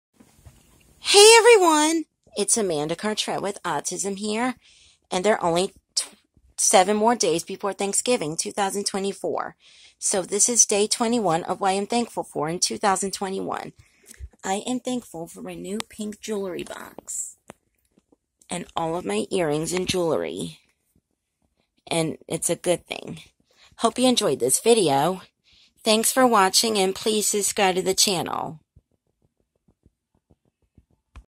What I Am Thankful For In 2024: Day 21
There Are 7 More Days Before Thanksgiving 2024. So This Is Day 21 Of What I Am Thankful For In 2024. I Am Thankful For My New Pink Jewelry Box And My Jewelry.